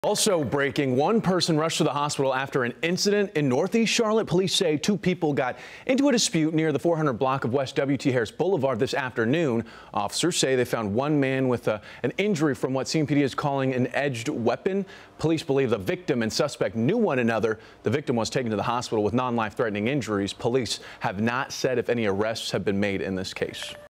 One injured in northeast Charlotte dispute
Police say two people got in a dispute near the 400 block of West W.T. Harris Boulevard Saturday afternoon. Officers say they found one man with an injury from what CMPD is calling 'an edged weapon.'